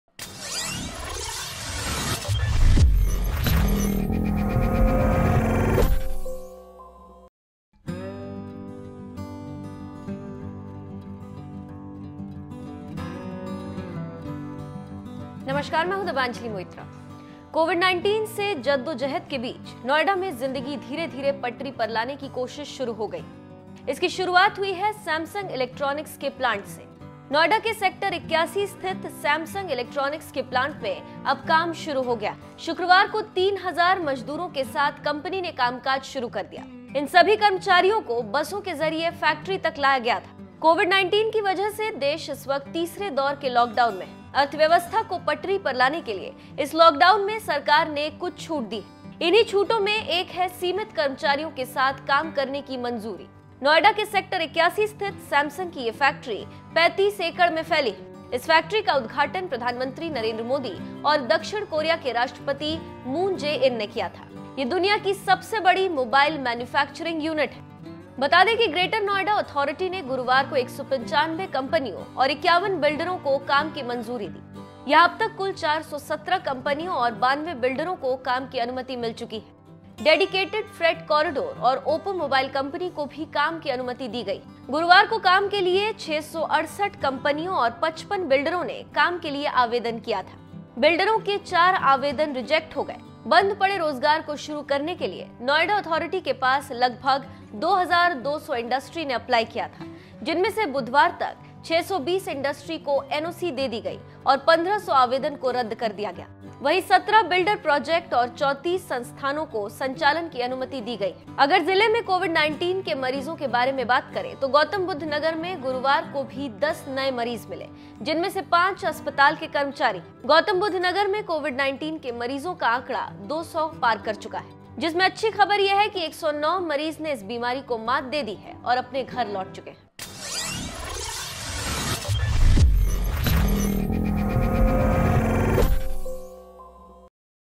Noida में दुनिया के सबसे बड़े Mobile Plant में 3000 कर्मचारियों के साथ काम शुरु
कोविड 19 से जद्दो जहद के बीच नोएडा में जिंदगी धीरे धीरे पटरी पर लाने की कोशिश शुरु हो गई है। इसकी शुरुआत हुई है सैमसंग इलेक्ट्रॉनिक्स के प्लांट से । नोएडा के सेक्टर 81 स्थित सैमसंग इलेक्ट्रॉनिक्स के प्लांट में अब काम शुरु हो गया है।